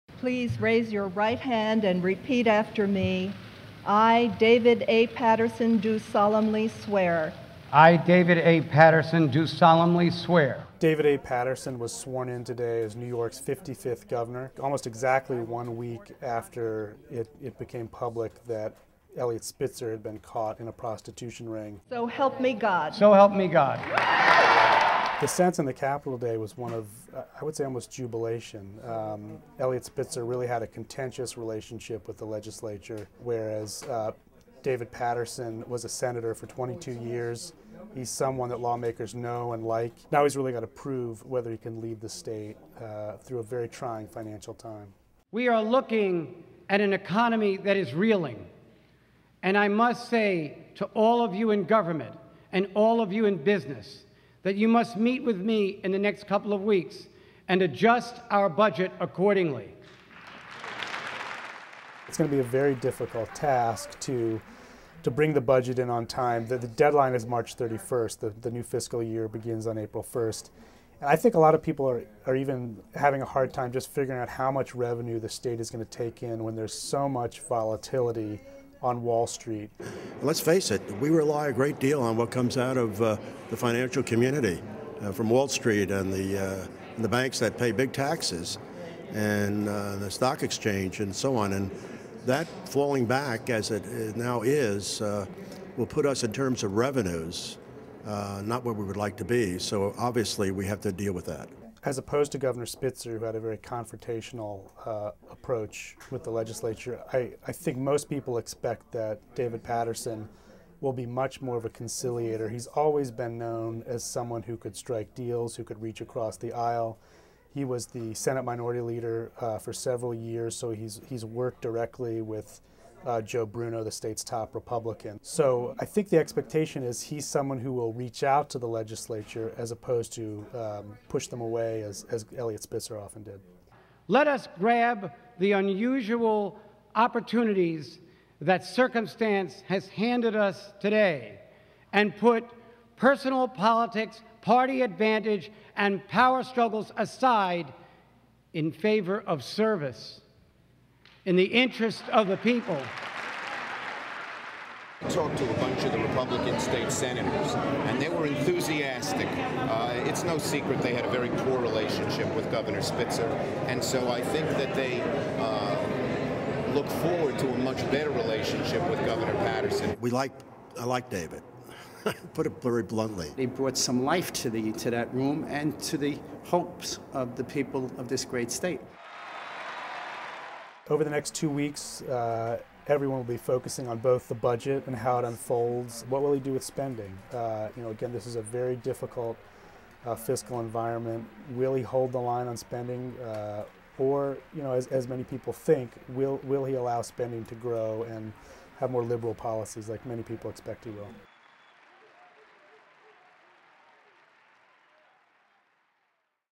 NYTimes.com - Fresh Challenges for Governor Paterson
NYTimes.com - New York Gov. David A. Paterson took office under pressure to balance the state's budget. Albany bureau chief Danny Hakim reports.